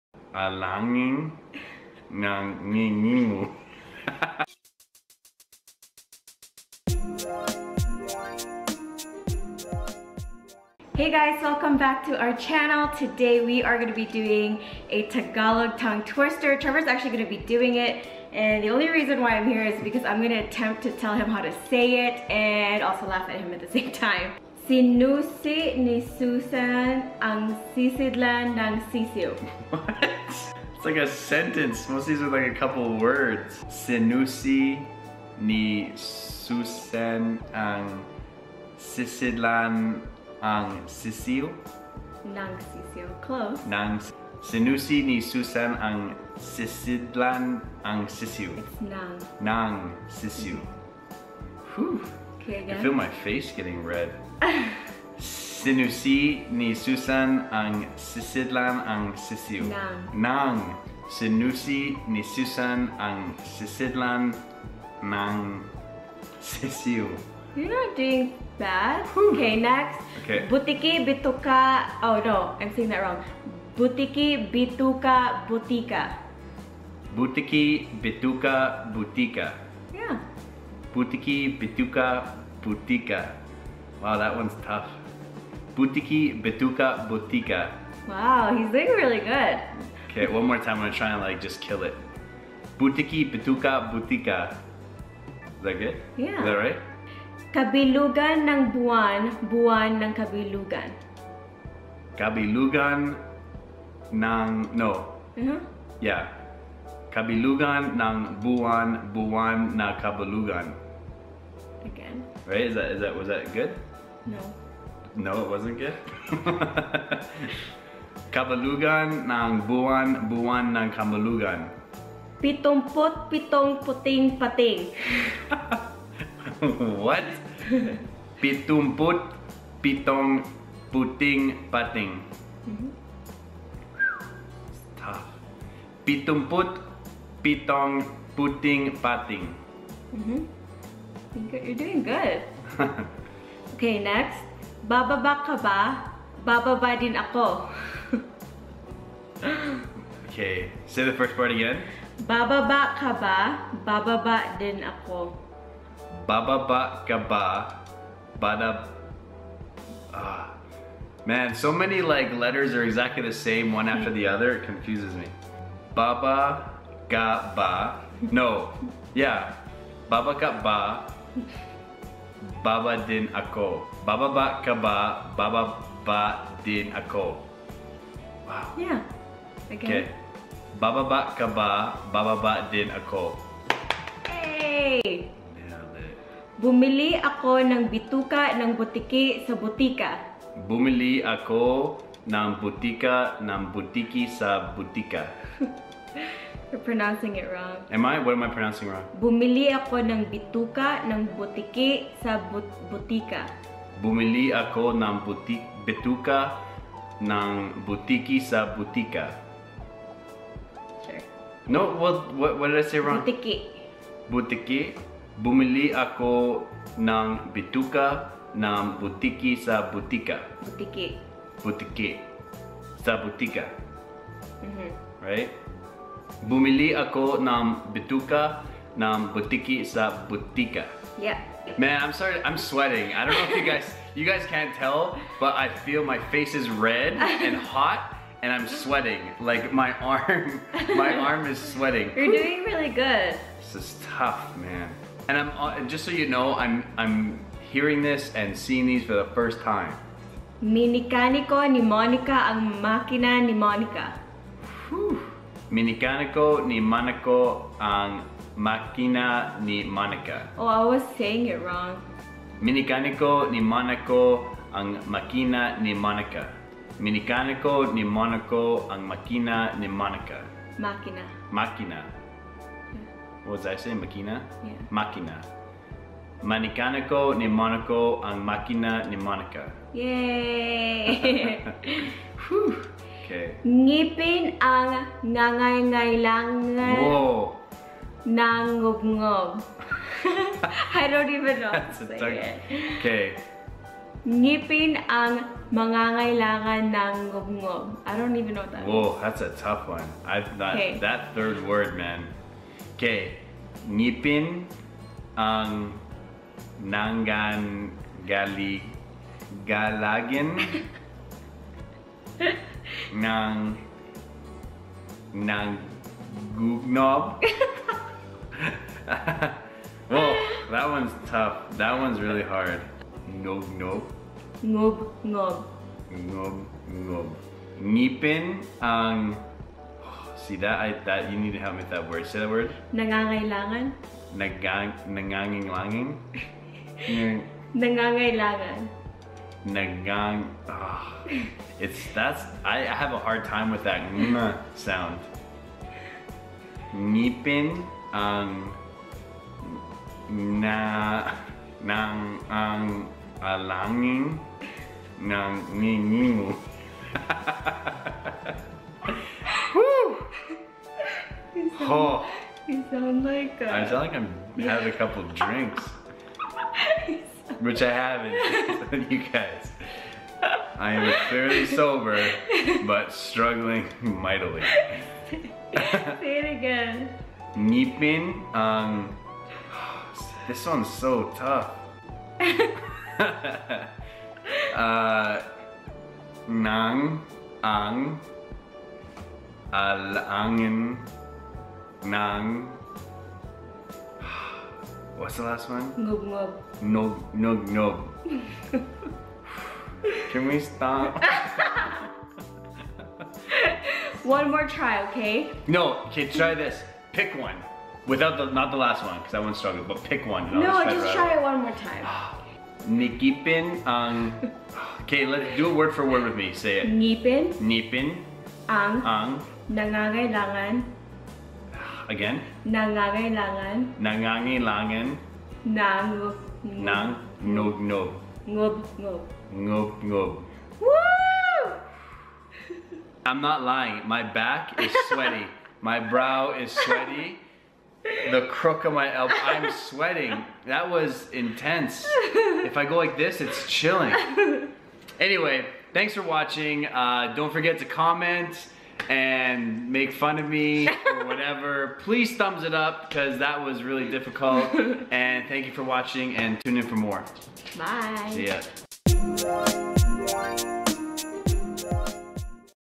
CANADIAN DOES TAGALOG TONGUE TWISTER
To those who can understand, hope you enjoy this Filipino Friday episode! Trevor actually did a really good job. I'm impressed!